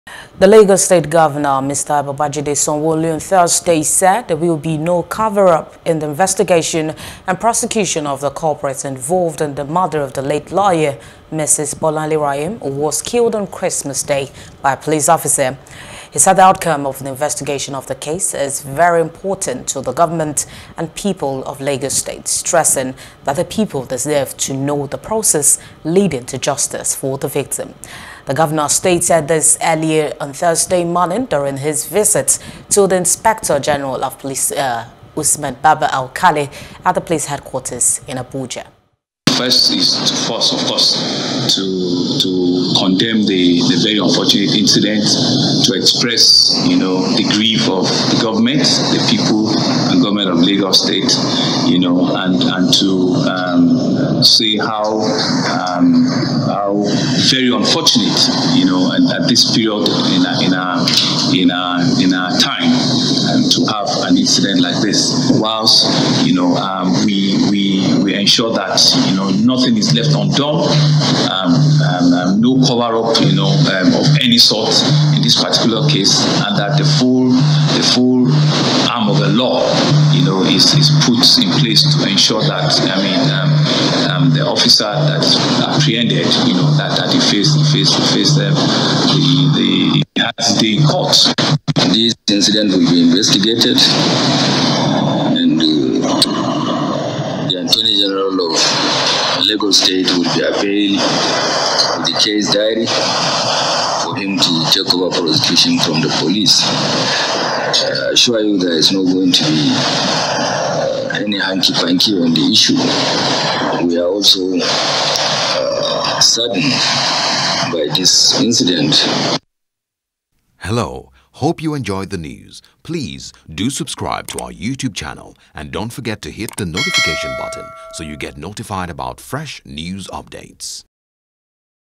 Bolanle Raheem: There Will Be No Cover-Up In Investigation-Sanwo-Olu
Lagos State Governor, Mr. Babajide Sanwo-Olu, on Thursday said there would be no cover-up in the investigation and prosecution of the culprit(s) involved in the murder of the late lawyer, Mrs. Bolanle Raheem, who was killed on Christmas Day by a police officer.